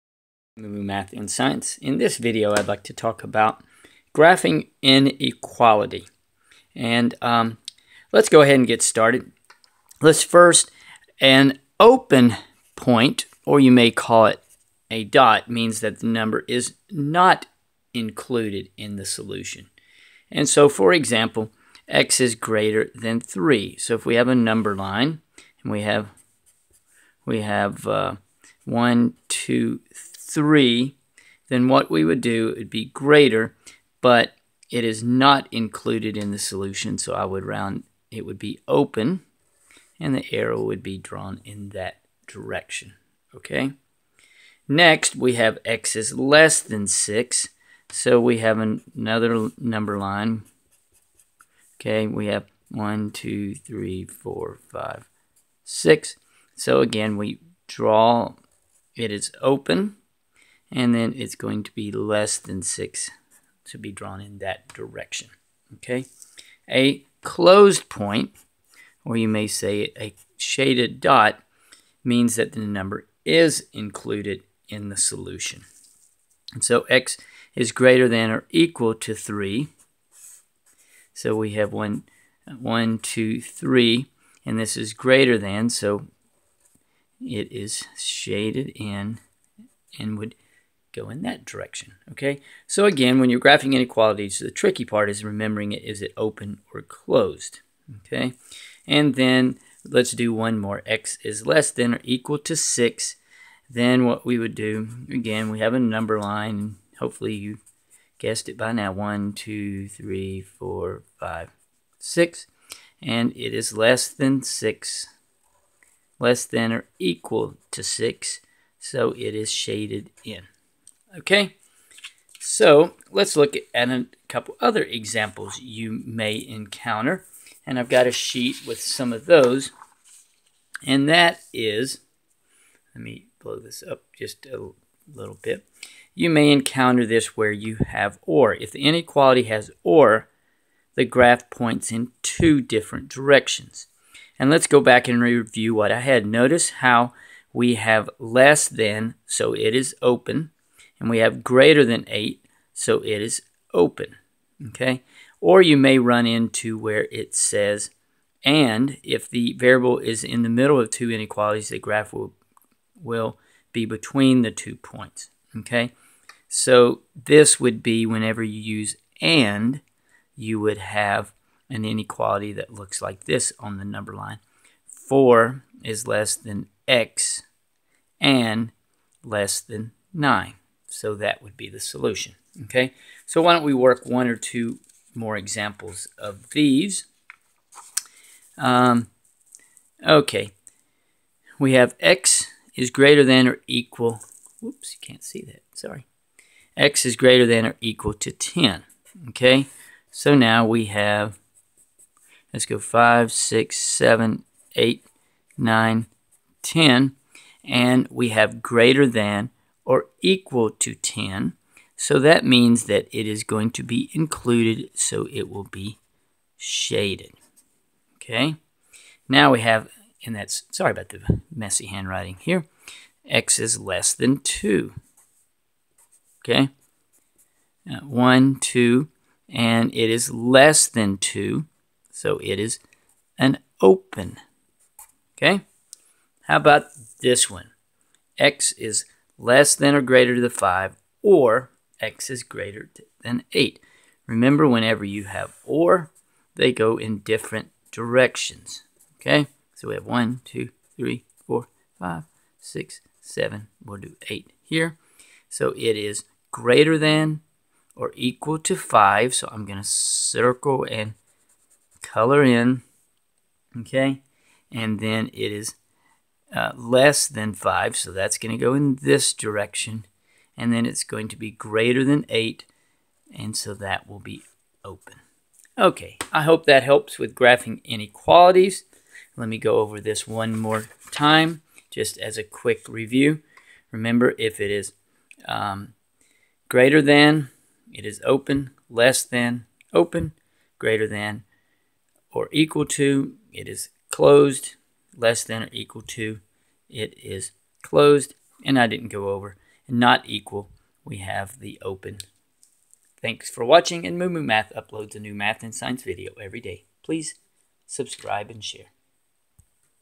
Graphing inequalities on a Number Line
Learn to graph inequalities on a number line.
Inequalities include less than, less than and equal to, greater than, and greater than and equal to.
If the inequality is greater than and equal to or less than and equal to then shade in the dot.
The dot is open when you have greater than or less than.
Inequalities on a number line resources